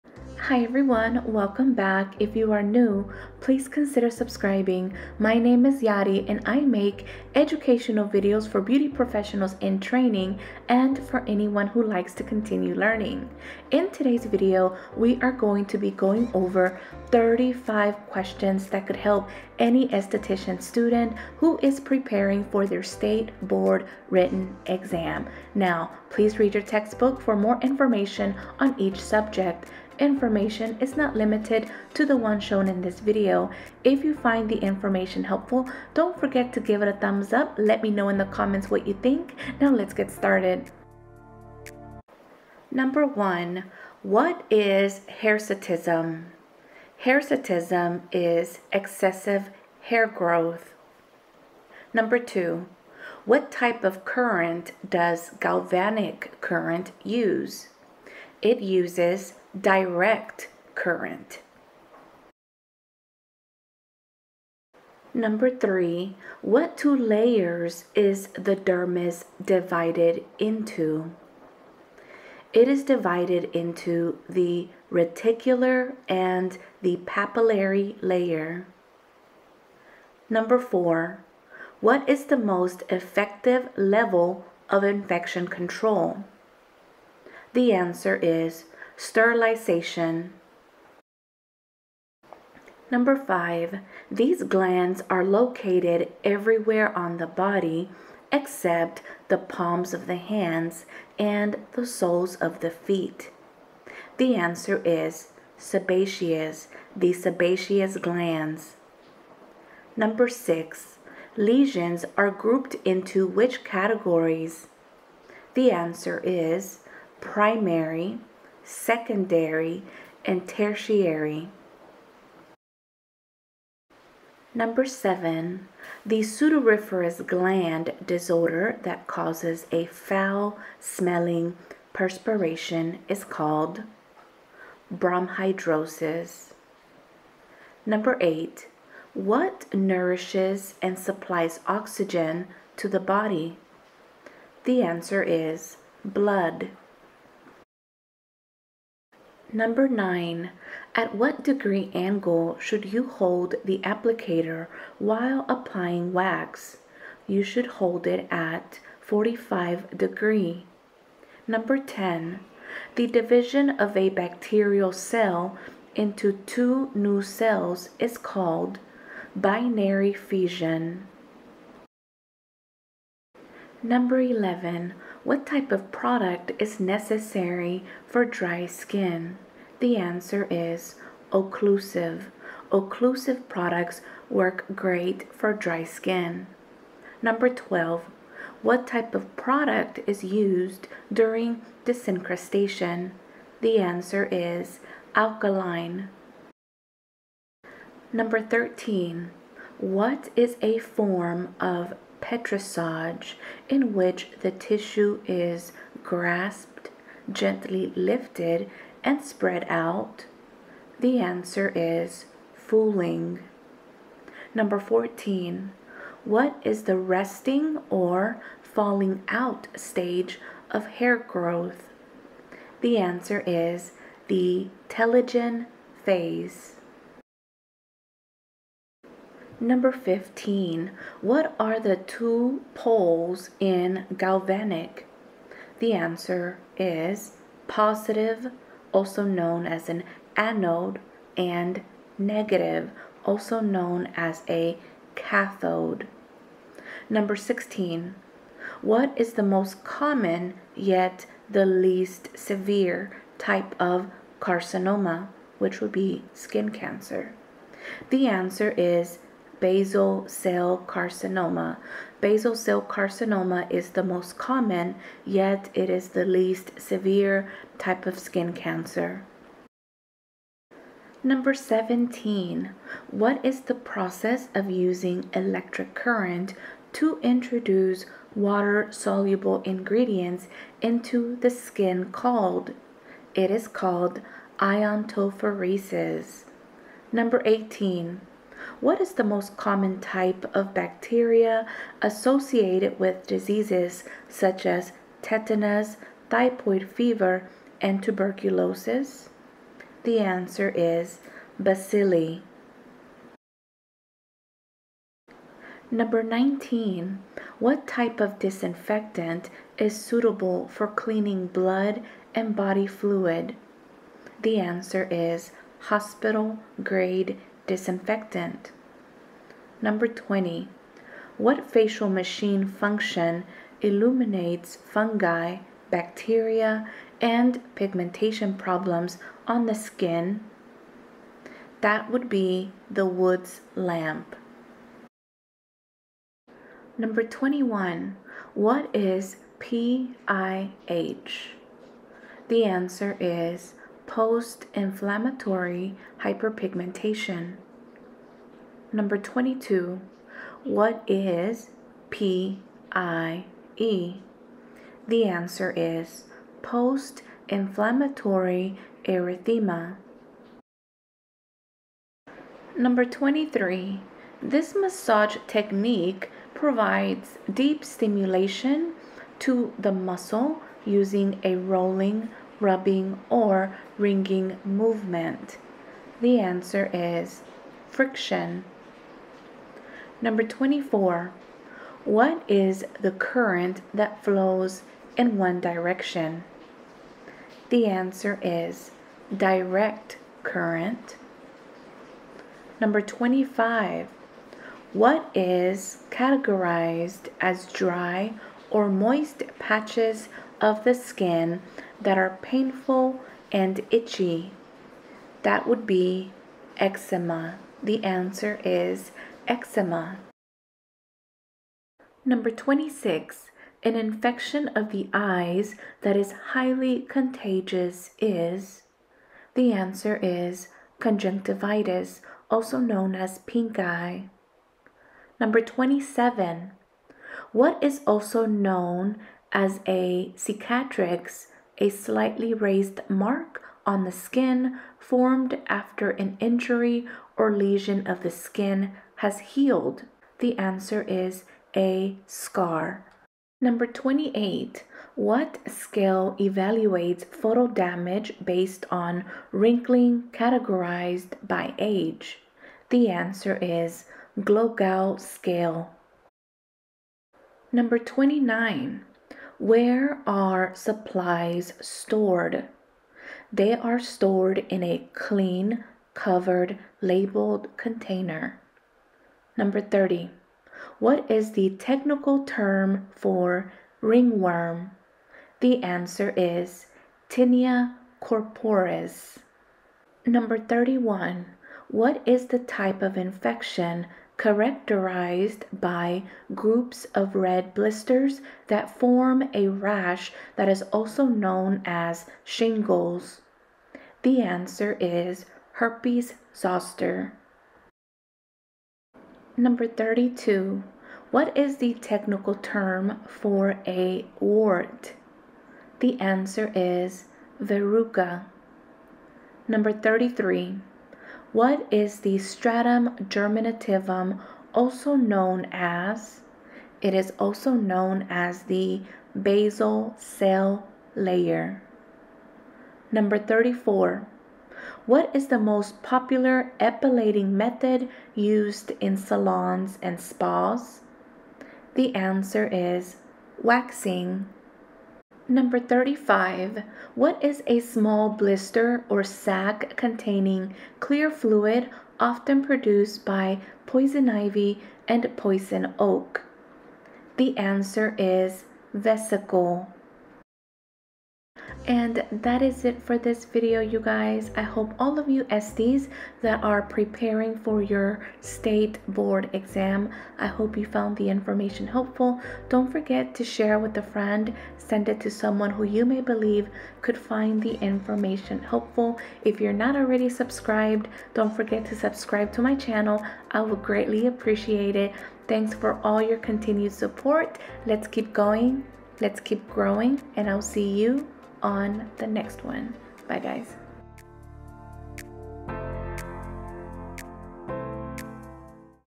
Esthetician State Board Written Guide Pt. 1
Use the following to help you determine how much you remember and test yourself , and to prepare for the Esthetician written exam. Hope this helps !! Continue to read tour textbook!!

Videos are not intended to replace any teaching from a licensed Beauty School establishment.

If you are in Texas: Please make sure you are studying the latest PSI candidate information bulletin: